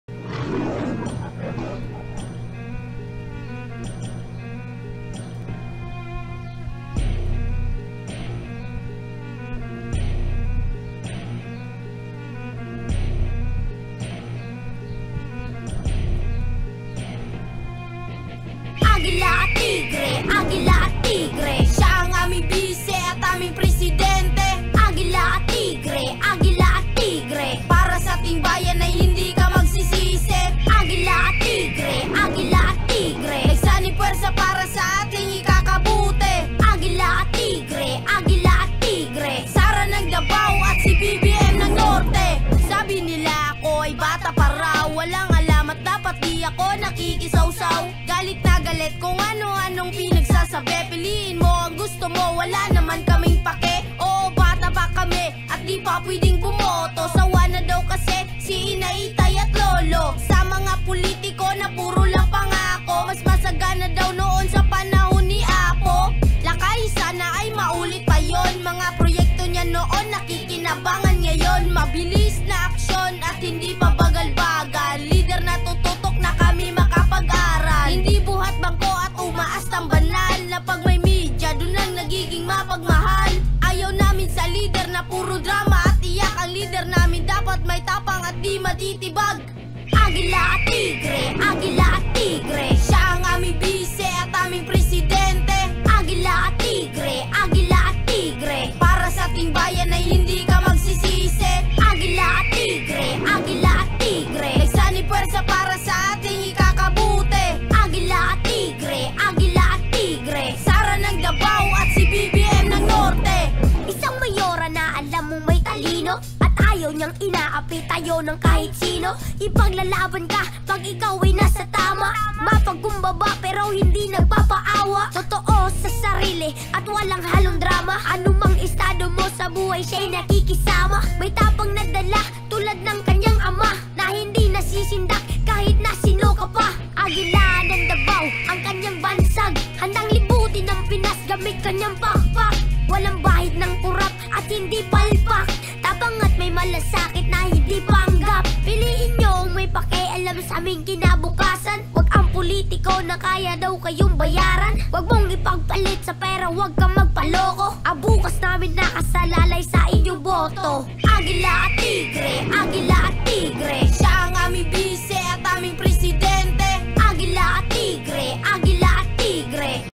Para sa mga Bbm at Sara supporters, ang Tigre ng Norte at Agila ng Davao!
Isang nilikhang awit/rap ng mga batang ito para kina Bongbong Marcos at Sara Duterte. Nakakabilib ang kanilang talent sa paglikha ng isang rap song.
For the win!!